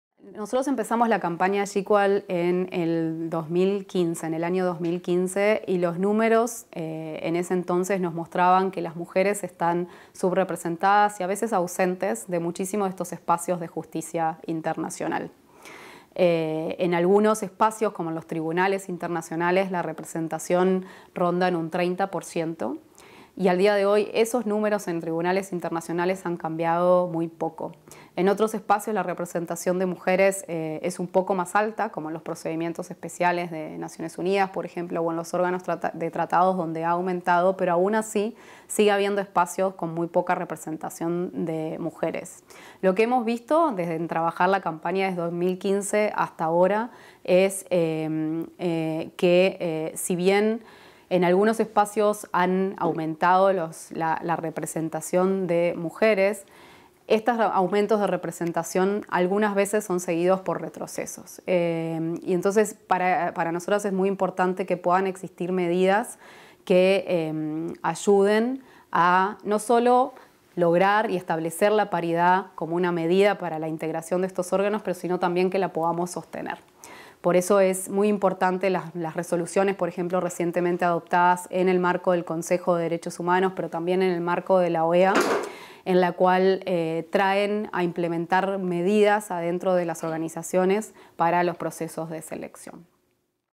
Campaña GQUAL y subrepresentación
María Noel Leoni, Directora de la Campaña GQUAL nos cuenta sobre el trabajo que realizamos desde el 2015 por alcanzar la paridad de género en los órganos y tribunales de justicia internacional y cómo han cambiado las cifras desde entonces.